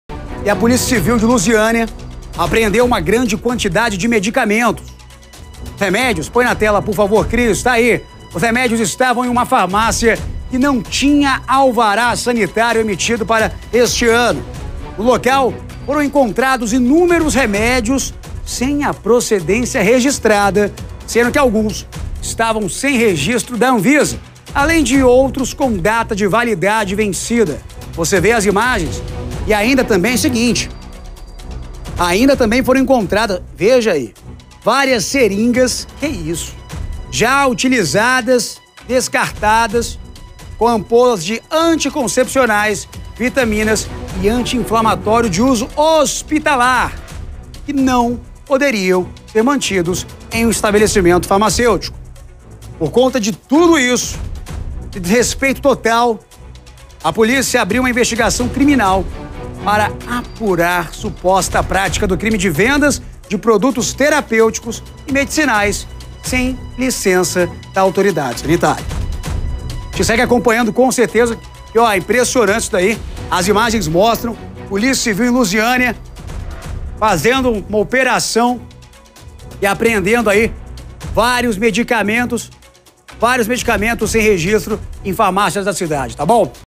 Polícia apreende remédios vencidos em farmácia de Luziânia (GO)
Remédios de uma farmácia em Luziânia, em Goiás, foram apreendidos após serem vendidos sem registro e fora do prazo de validade. De acordo com a Polícia Civil, também foram encontradas seringas usadas e medicamentos hospitalares. A corporação ainda investiga o caso.